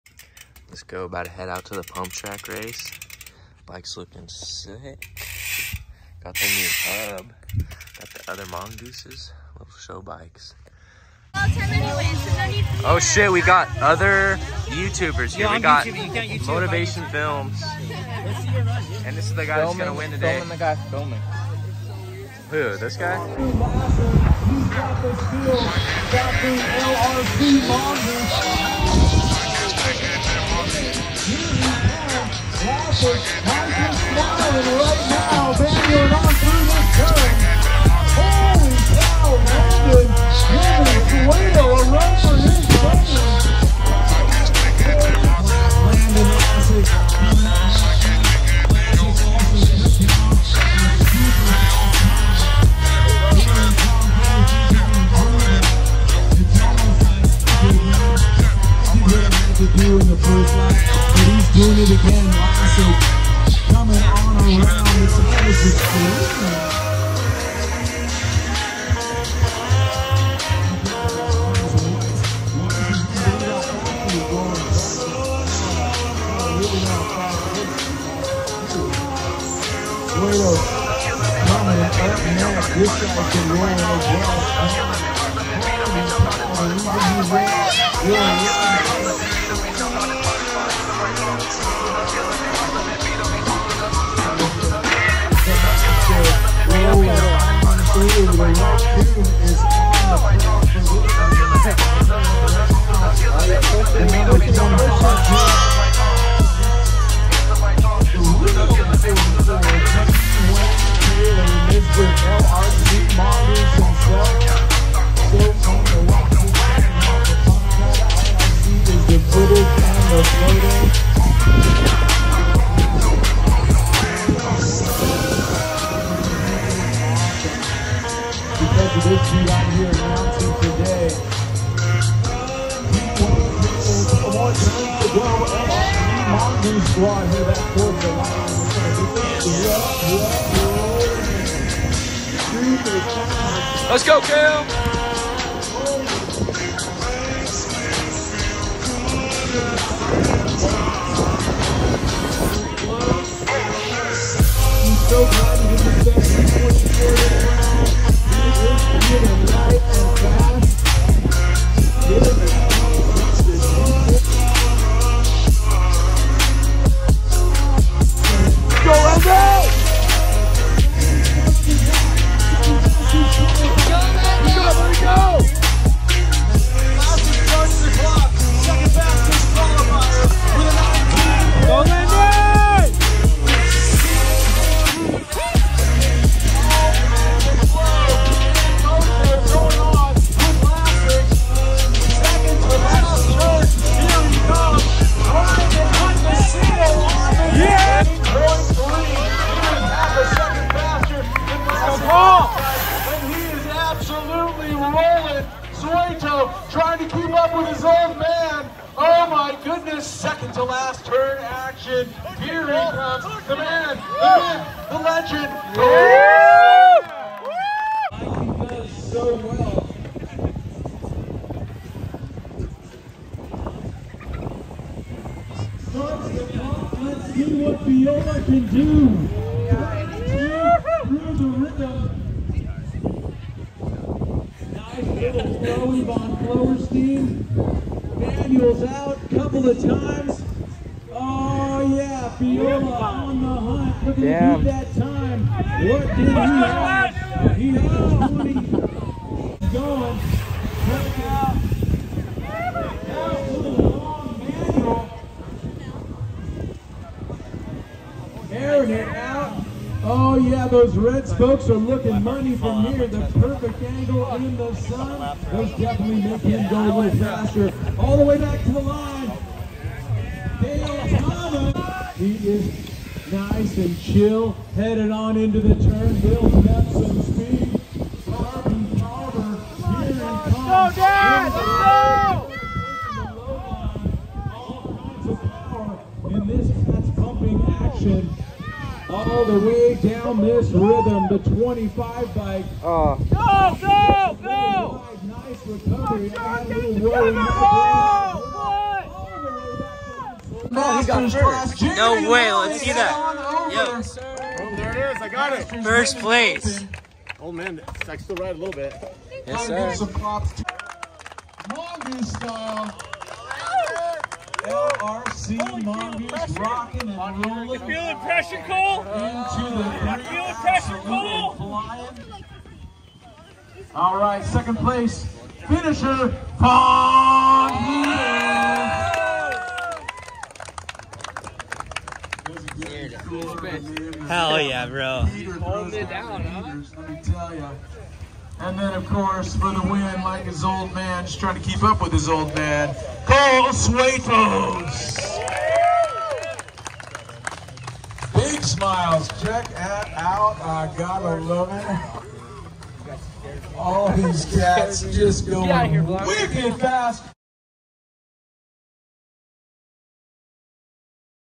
2022 USA Pump track championship race at Temecula pump track
sick time got 2nd in qualifying round and 3rd in finals